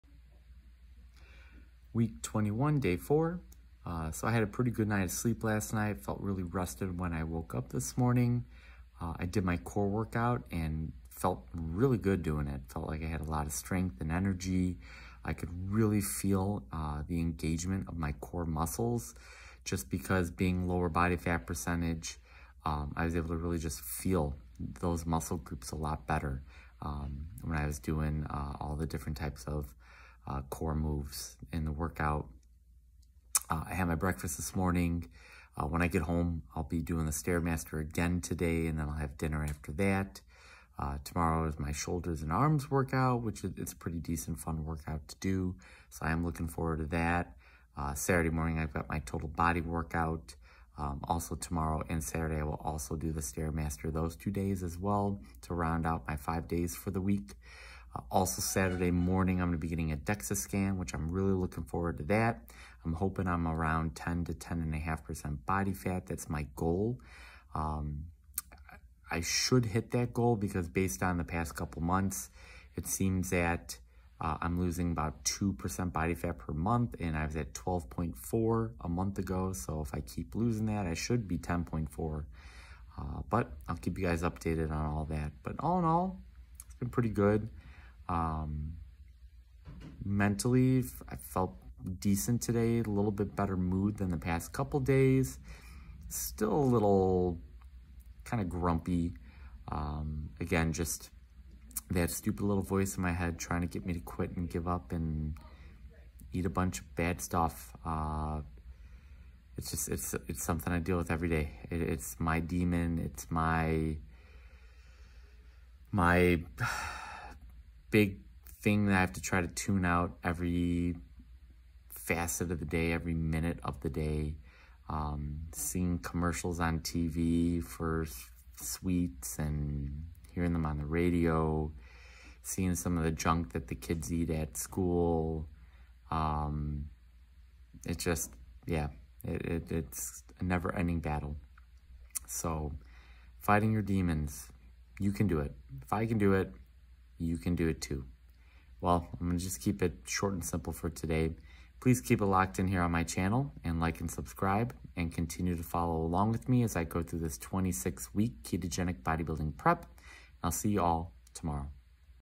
Week 21, Day 4
I had a pretty good night of sleep last night. I had a really good core workout this morning. I was able to really feel all the muscle groups and engage all the muscle groups of my core. I think this is because my body fat percentage is getting low enough now where I can really feel that particular muscle group. I’ll be doing the stairmaster when I get home. In a couple of days I’ll be getting another DEXA scan. My goal is to be between 10 and 10 1/2% body fat.